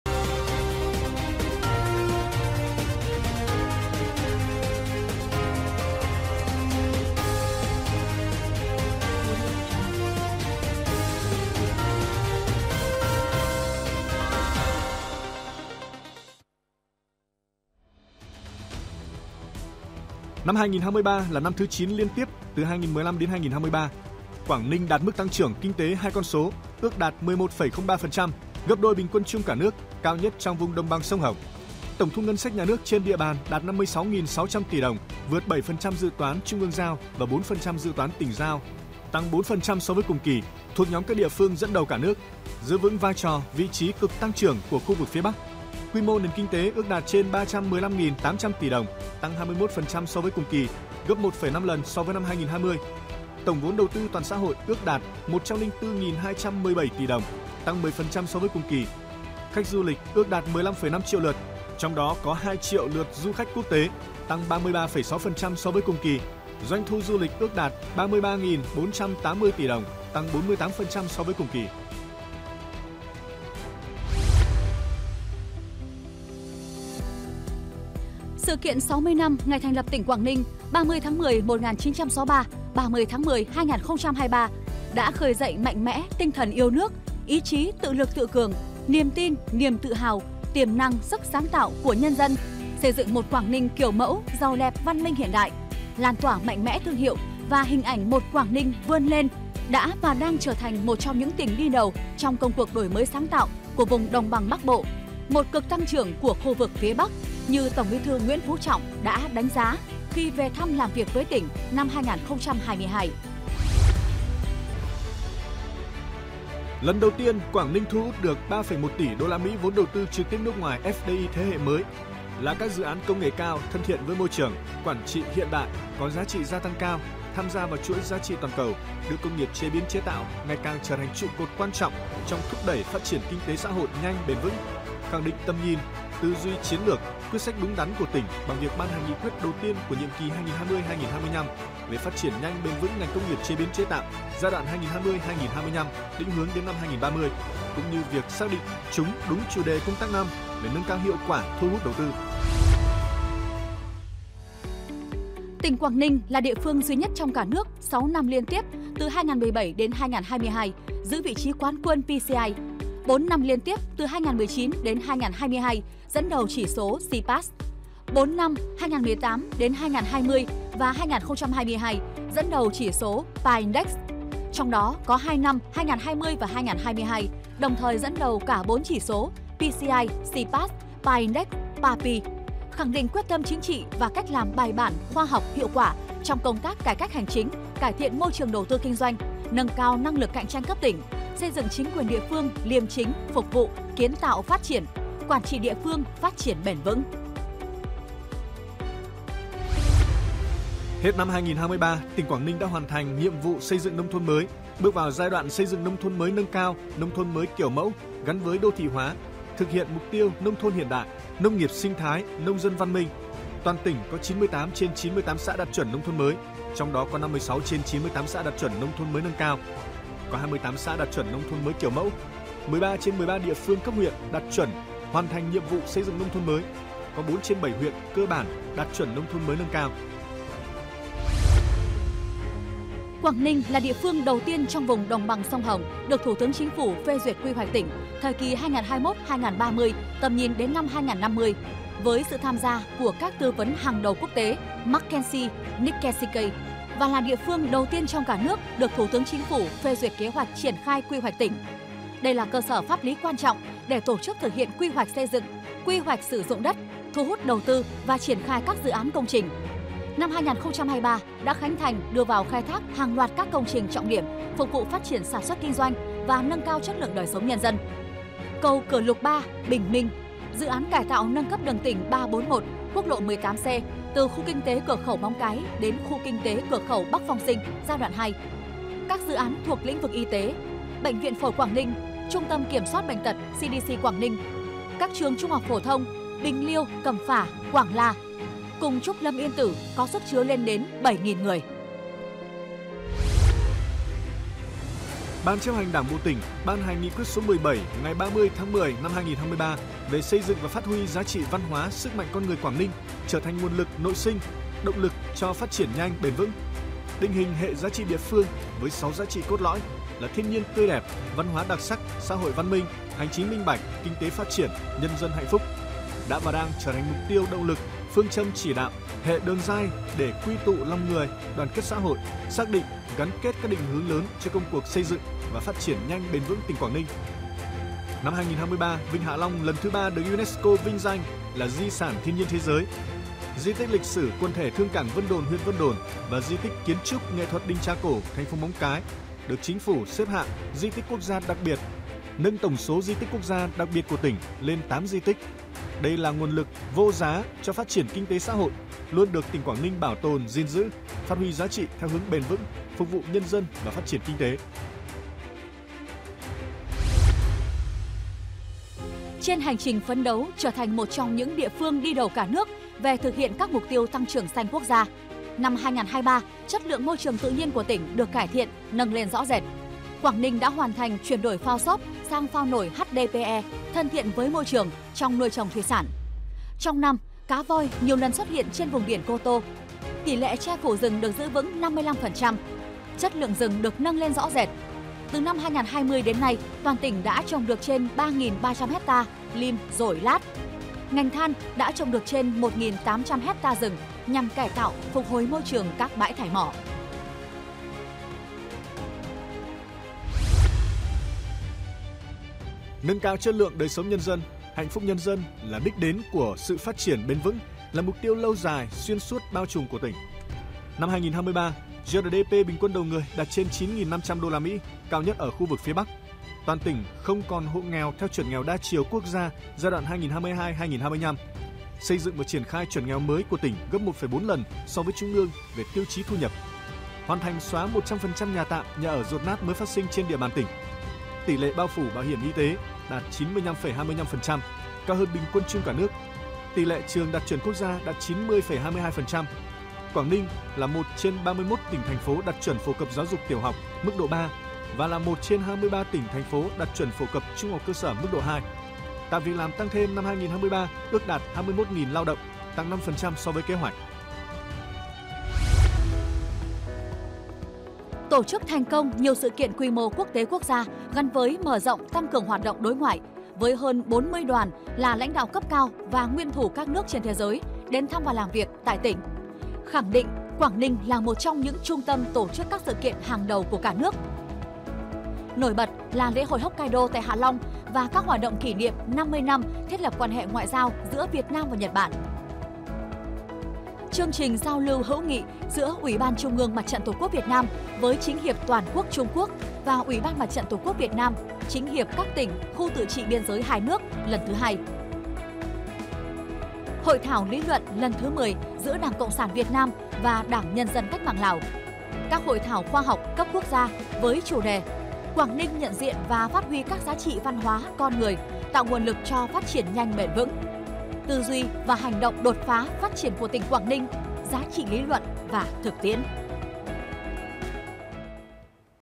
10 sự kiện tiêu biểu tỉnh Quảng Ninh 2023
Năm 2023, trong bối cảnh có rất nhiều khó khăn, thử thách chưa từng có từ đầu nhiệm kỳ tới nay, Đảng bộ, chính quyền, nhân dân các dân tộc tỉnh Quảng Ninh đã làm lên kỳ tích của tinh thần đoàn kết, thống nhất, kiên định, nhất quán, kiên cường vượt khó thành công, hoàn thành toàn diện các mục tiêu, chỉ tiêu, nhiệm vụ, chủ đề công tác năm “Nâng cao hiệu quả thu hút đầu tư và chất lượng đời sống Nhân dân” đề ra, giữ vững đà cải cách đổi mới sáng tạo vì hạnh phúc nhân dân bằng tinh thần và sức mạnh “Kỷ luật và Đồng tâm”, bằng giá trị văn hóa và sức mạnh của con người Quảng Ninh.